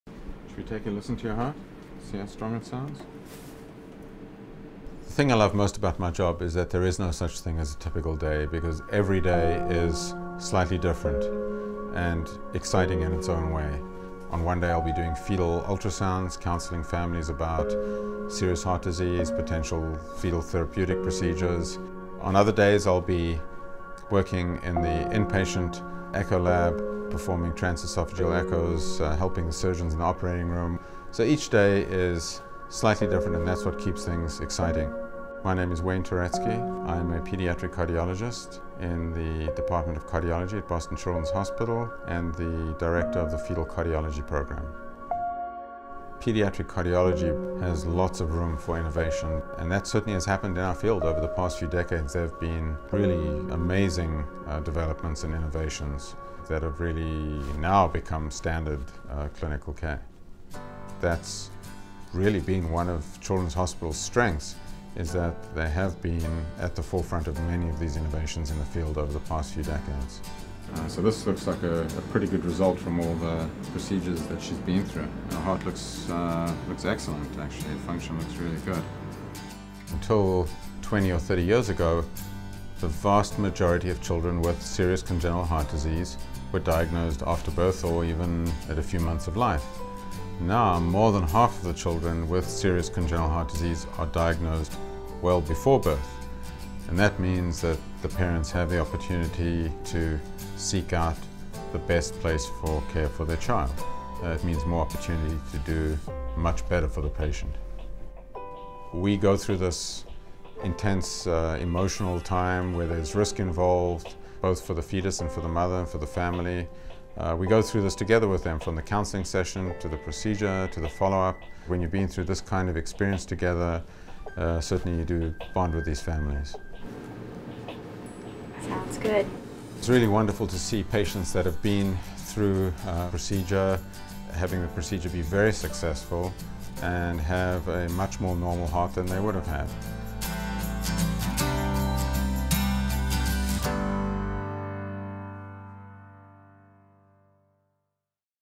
Cardiac Caregiver: Wayne Tworetzky, MD - Boston Children's Hospital
Pediatric Cardiologist Excited By Opportunity to Help Babies with Heart Defects

Wayne Tworetzky, MD, director of the Fetal Cardiology Program, explains how there is never a dull moment to making sure that babies with heart defects receive the best possible care. From the ongoing innovations at Boston Children's to developments in imaging techniques, the ability to treat these patients is only improving each day.